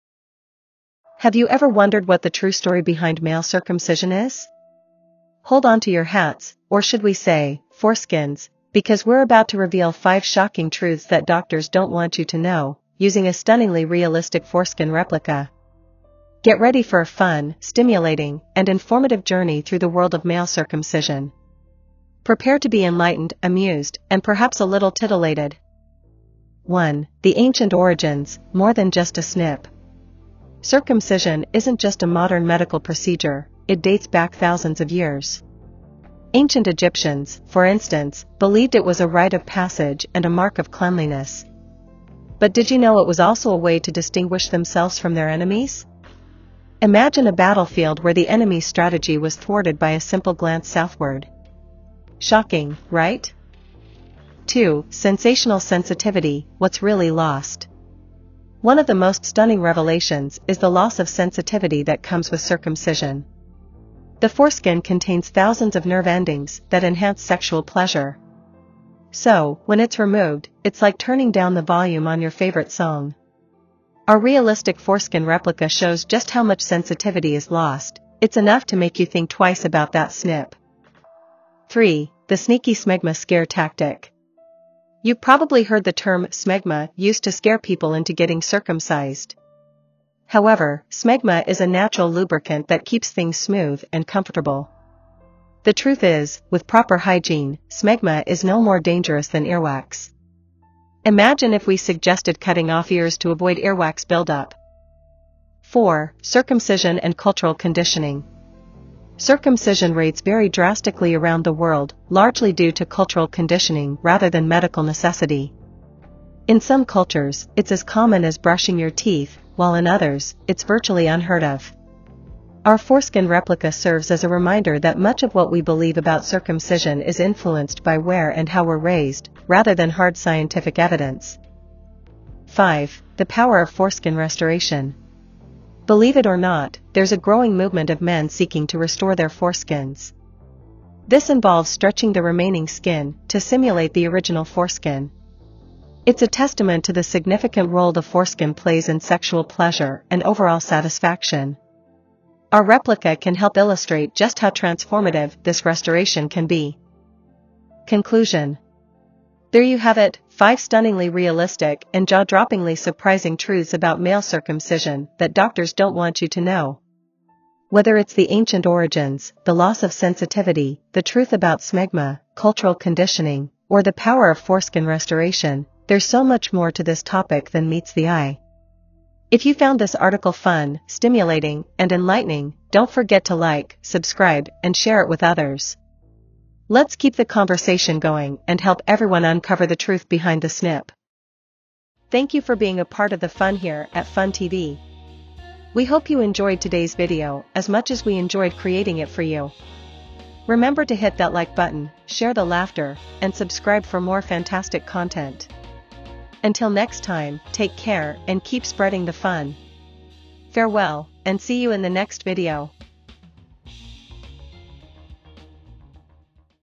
5 Shocking Truths About Male Circumcision Doctors Don't Want You to Know
5 Shocking Truths About Male Circumcision Doctors Don't Want You to Know

We talk about the surprising truths behind male circumcision that doctors don't want you to know. From the ancient origins of the practice to the loss of sexual sensitivity, the truth about smegma, cultural conditioning, and the growing movement of foreskin restoration, this video covers it all using a stunningly realistic foreskin replica.